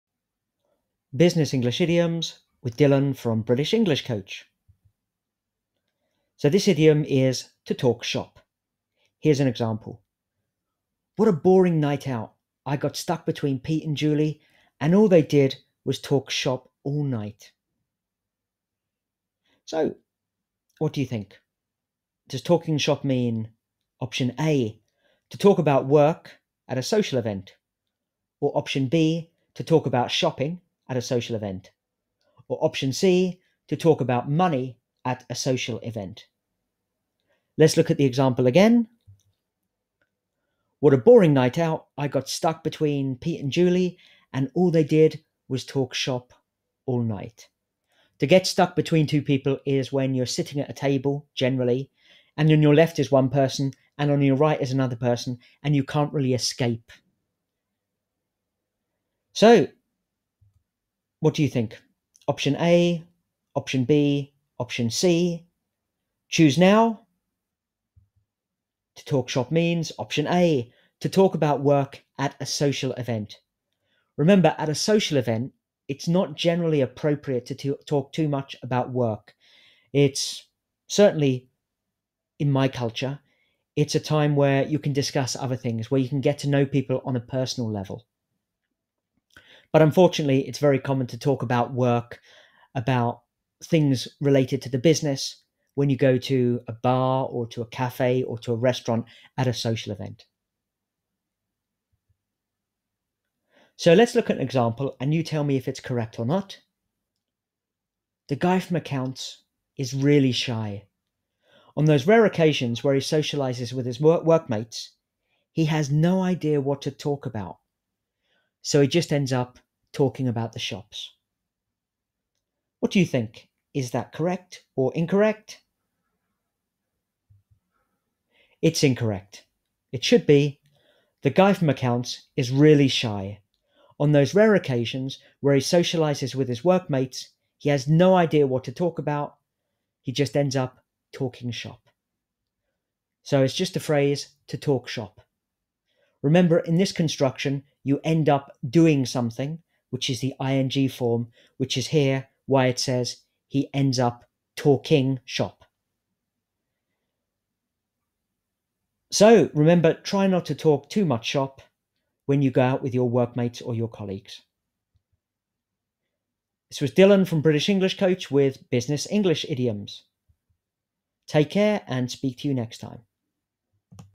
Business English Idioms - To talk shop
What happens when people talk shop? Find out here.